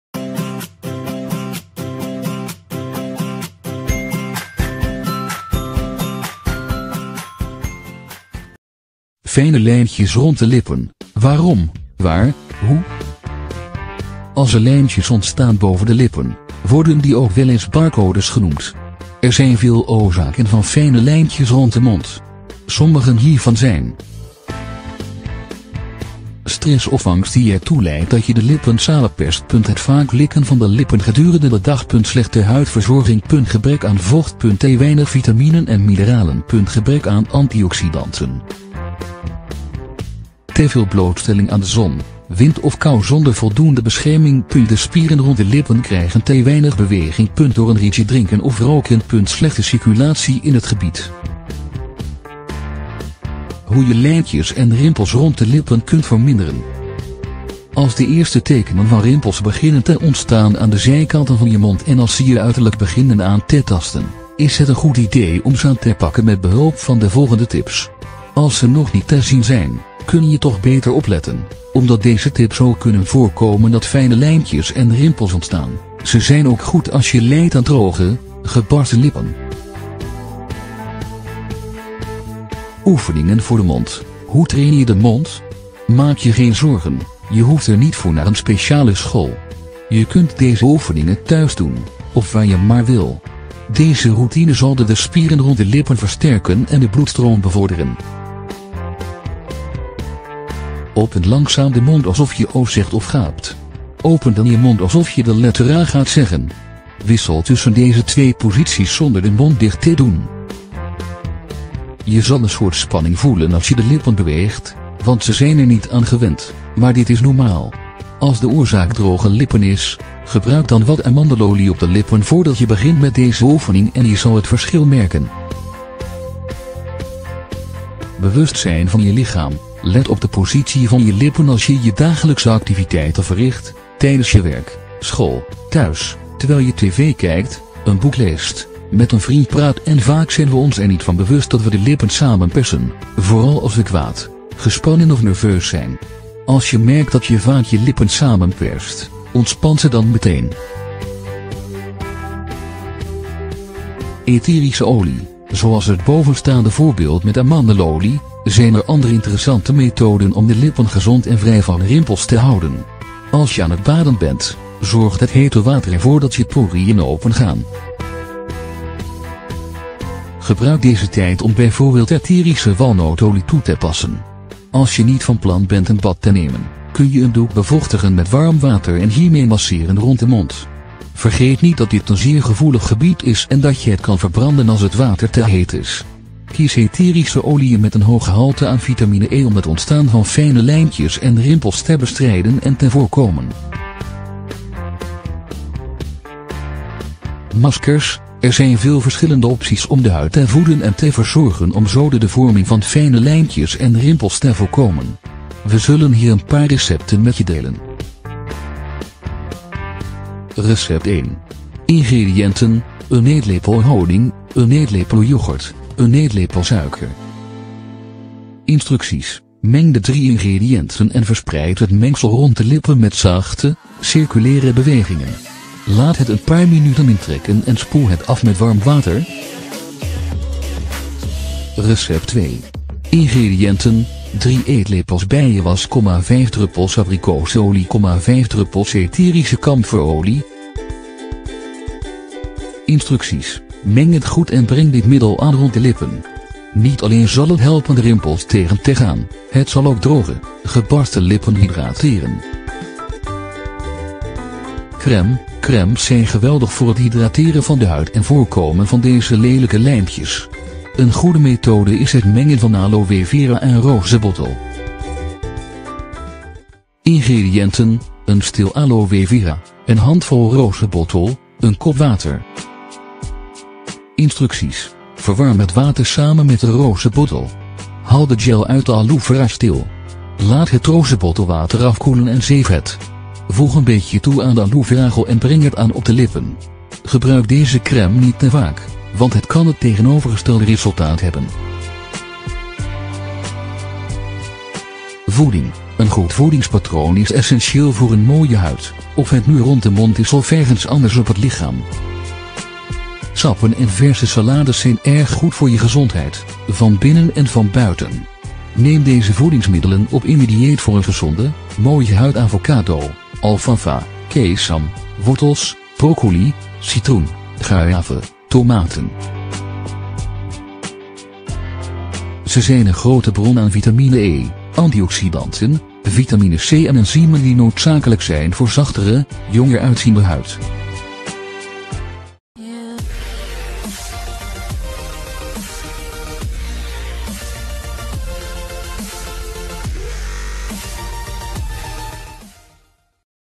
Het voorkomen van fijne lijntjes en rimpels
De lippen zijn omringd door een gevoelige huidlaag (net als de huid rond de ogen). Daardoor ontstaan rimpels en fijne lijntjes in dit gedeelte van het gezicht meer dan in andere gebieden. Blijf lezen om te leren hoe je fijne lijntjes en rimpels kunt voorkomen,

Fijne lijntjes rond de lippen: waarom, waar, hoe
Rimpels
Als er lijntjes ontstaan boven de lippen, worden die ook weleens ‘barcodes’ genoemd. Er zijn veel oorzaken van fijne lijntjes rond de mond. Sommigen hiervan zijn:

Stress o